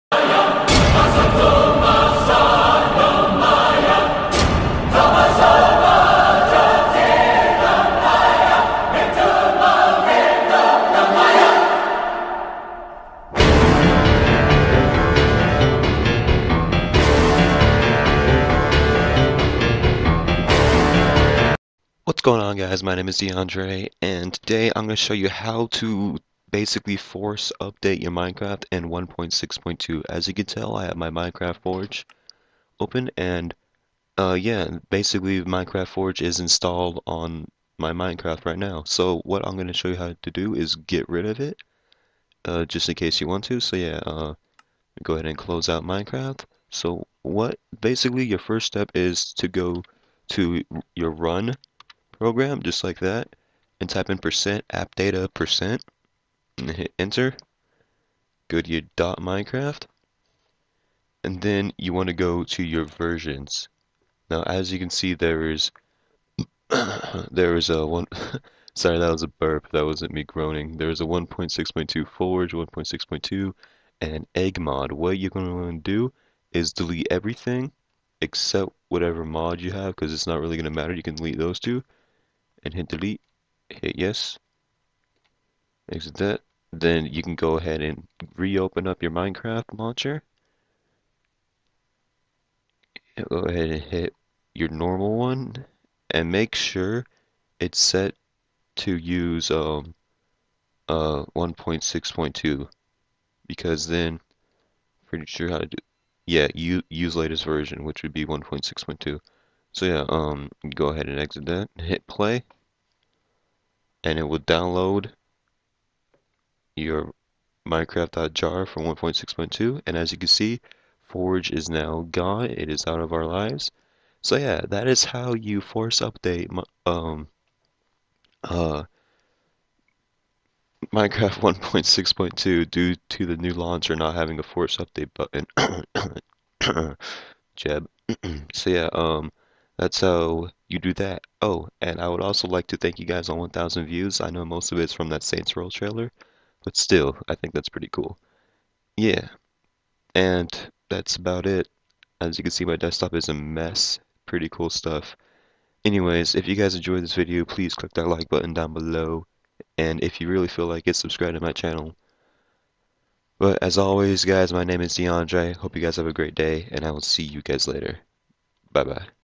How to force update Minecraft with the new launcher!!! (1.6.2)
Hey guys, for those of you who played Minecraft back in the olden days you know this process was much simpler, but the great minds at team Mojang have decided to give us a challenge. It's actually not that hard, and it works. If you guys enjoyed this video and/or found it useful please click that like button down below. As always my name is DeAndre, I hope you have a great day, and I'll see you later :)